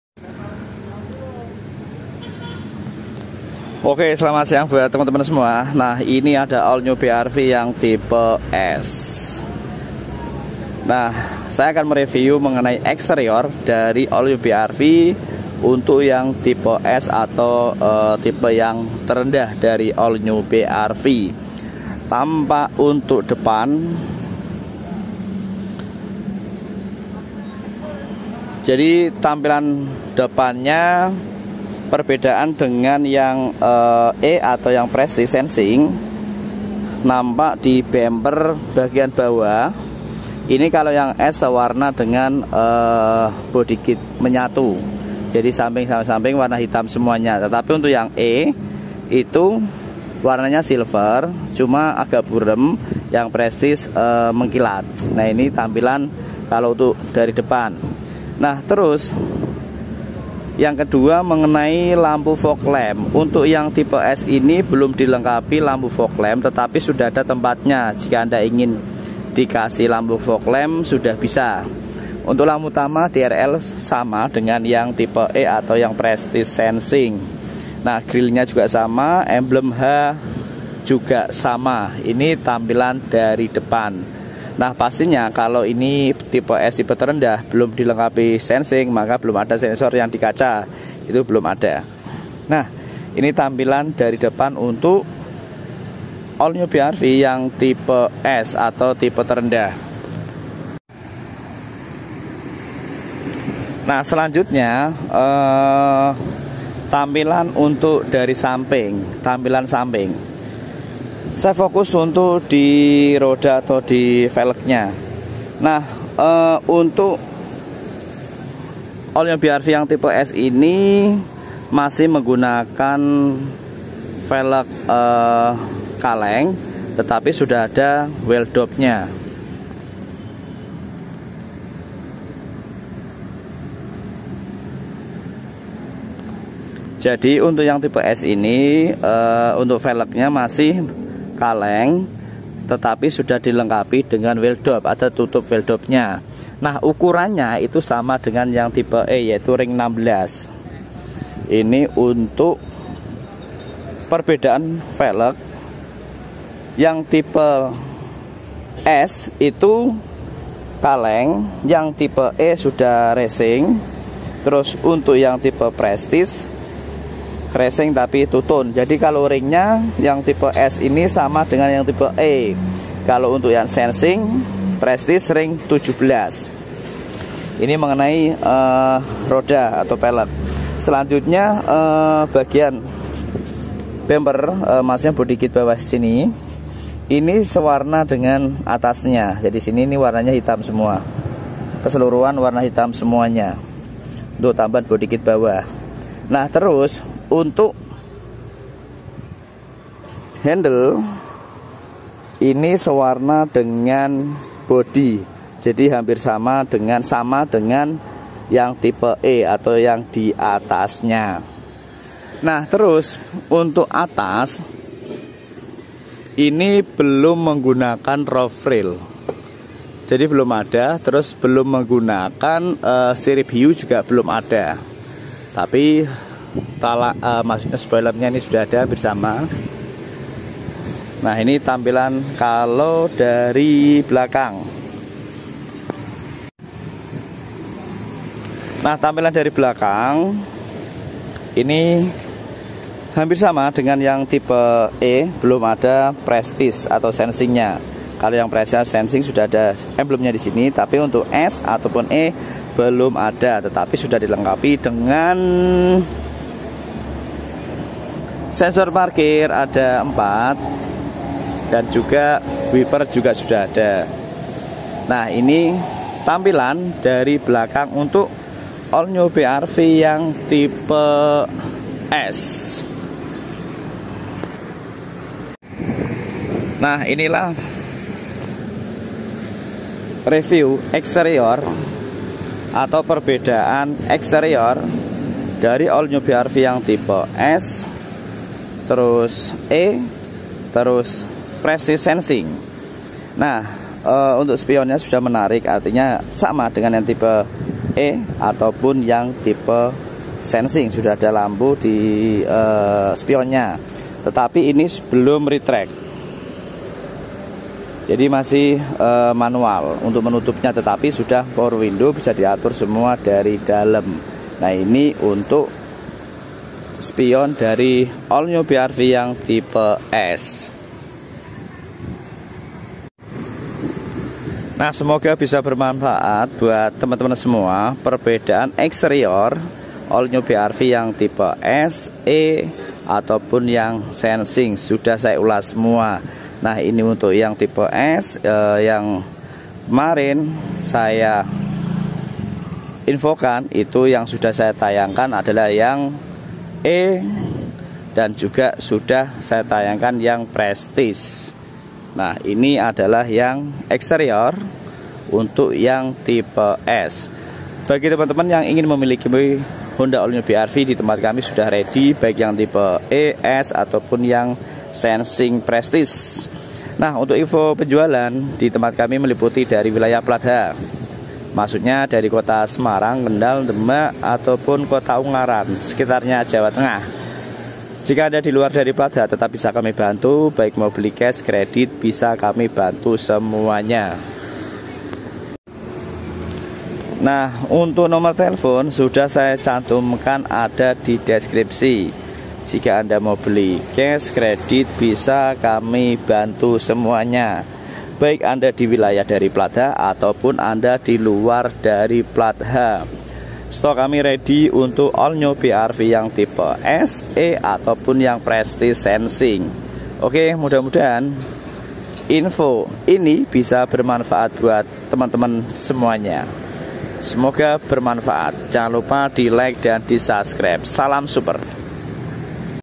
REVIEW ALL NEW BRV S MANUAL EXTERIOR | TIPE TERENDAH MODERN STEEL METALLIC
REVIEW ALL NEW BRV S MANUAL EXTERIOR | TIPE TERENDAH @HondaIndonesia

Info penjualan cash kredit
Honda kusuma Semarang
Outlet kendal Jawa Tengah